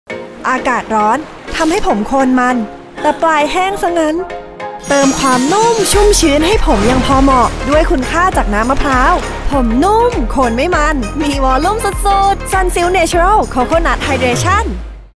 ผมนุ่มโคนไม่มัน มีวอลลุ่มสุดๆด้วยซันซิล เนเชอรัล โคโคนัทไฮเดรชั่น
ซันซิล เนเชอรัล โคโคนัทไฮเดรชั่น แชมพูใสสูตรธรรมชาติ พร้อมคุณค่าน้ำมะพร้าวจากธรรมชาติ 100% เติมความชุ่มชื้นให้เส้นผมอย่างพอเหมาะ ให้ผมนุ่มมีวอลลุ่ม โคนไม่มัน